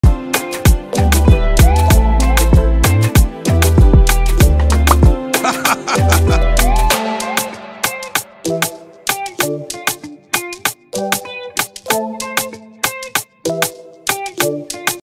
Sea cucumber’s 🥒 embarrassing photos
My sea 🌊 cucumber 🥒 is tossed into the air (water) by my tomato 🍅 clownfish’s wave attack 🤣🤣🤣

This was actually a clip from my hermit eating crab 🦀 episode 🤭🤭🤭😊😊😊🤣🤣🤣